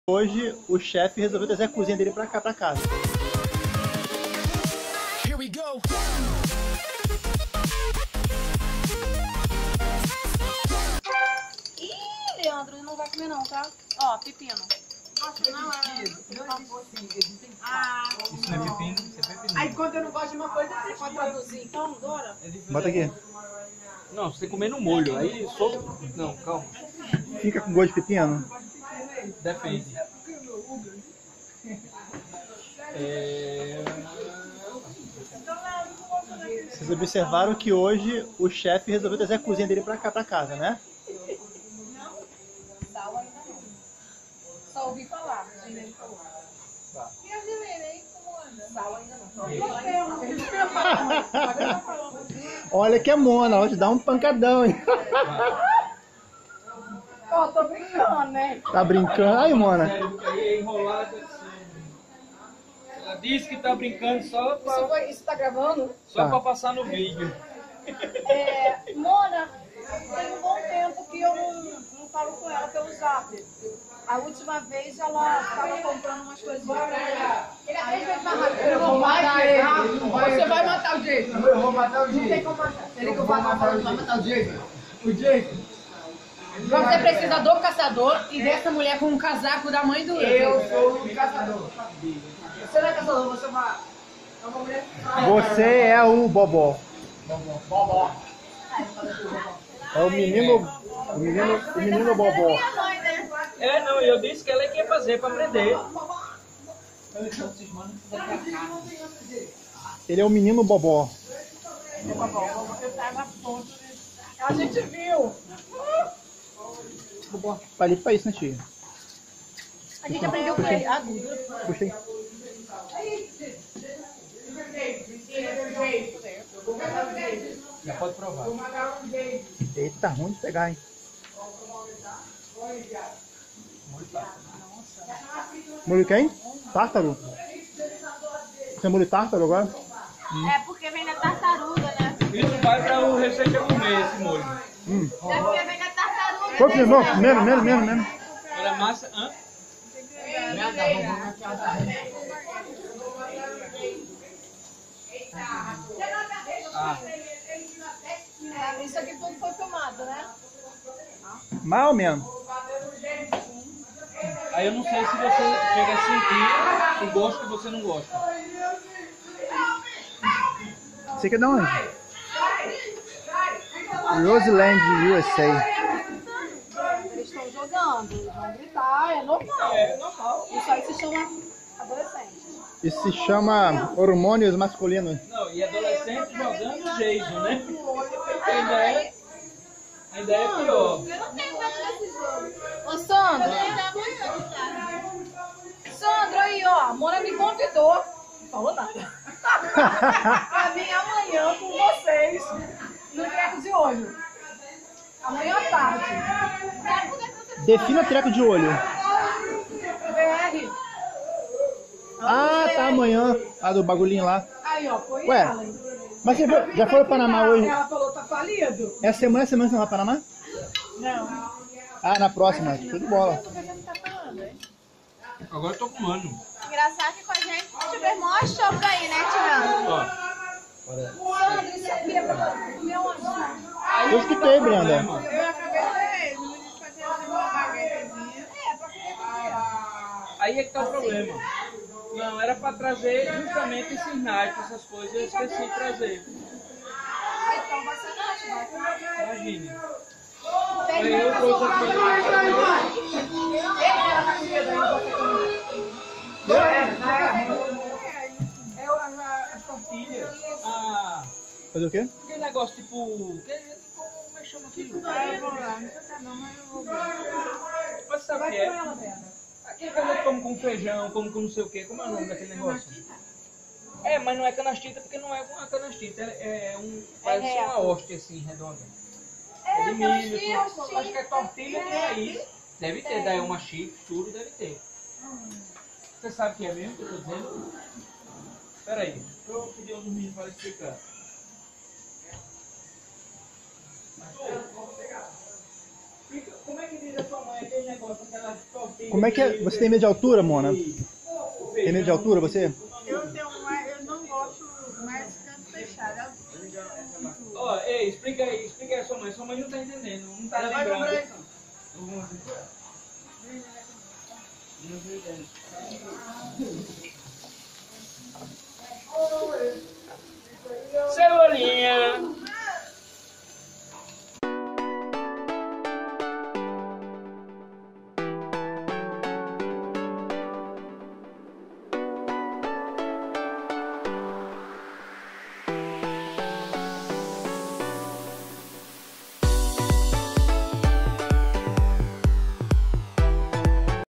#045 UM CHEF EM NOSSA CASA
como de costume toda semana nos reunimos para por as conversas em dia . só que dessa vez galera não contávamos com um chef em nossa casa.